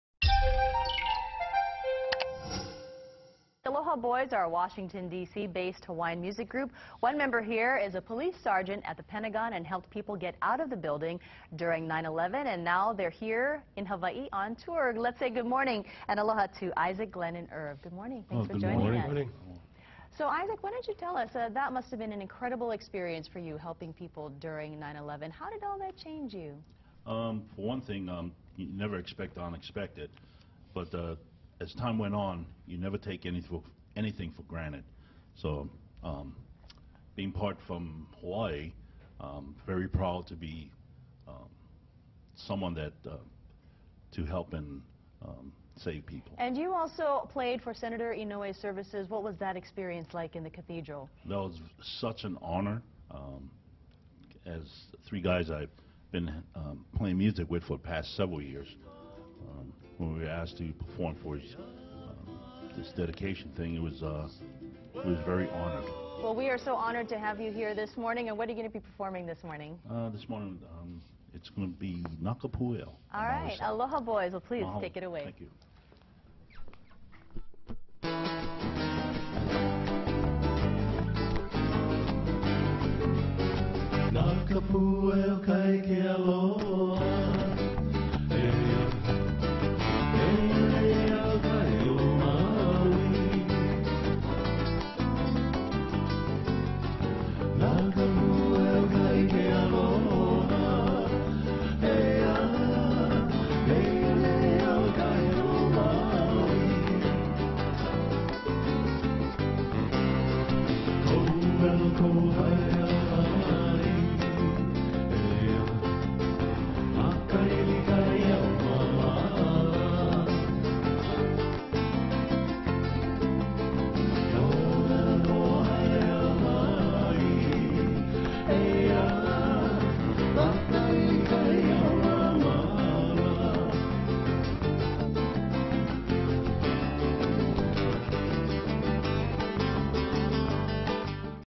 Aloha Boys on tour in Hawaii
The Aloha Boys are a Washington DC based music group who spread the local spirit at our nation's capitol, and recently at Senator Inouye's memorial service. They're here on the island's with a Hawaii tour, and stopped by KITV4 Morning News with a pr For news you need to know,